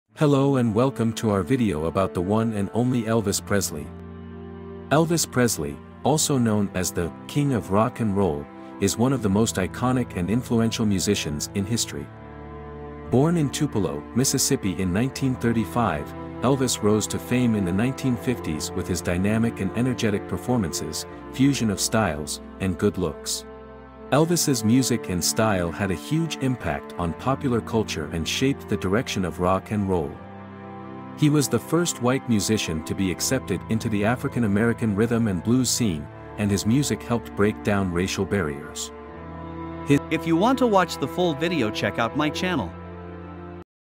Elvis Presley: The King of Rock and Roll
Join us on a journey through the life and career of Elvis Presley, the "King of Rock and Roll." From his humble beginnings in Tupelo, Mississippi to his rise to fame in the 1950s and beyond, we'll explore Elvis's iconic music and style, his impact on popular culture, and his acting career. We'll also touch on the tragic end of Elvis's life and the enduring legacy he left behind. This video is a must-see for any Elvis fan or anyone interested in the history of rock and roll.